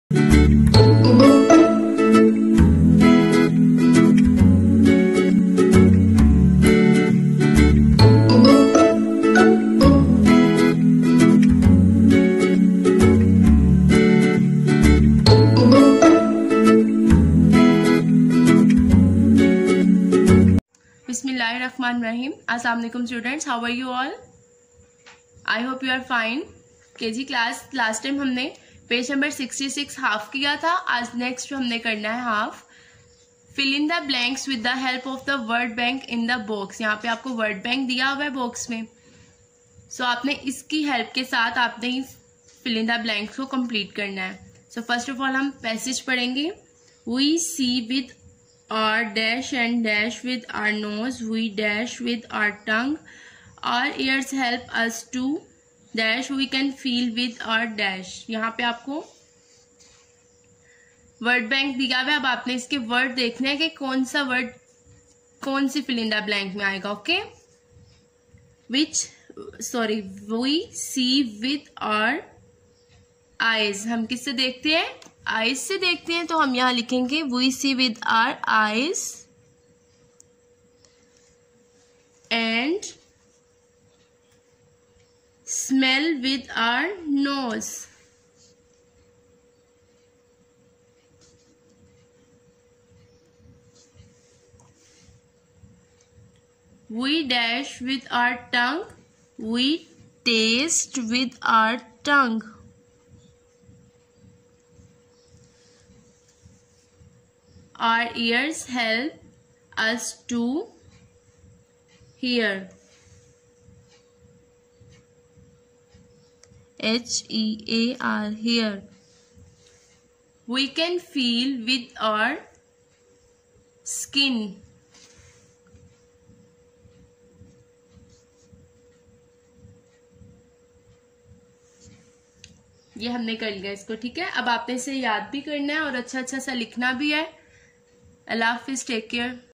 Summer Pack-Pg#66 (Next Half)
Subject: Science
Topic: Summer Pack-Pg#66(Next Half)
Level: Kindergarten
Lecture By: Ms Moon Tariq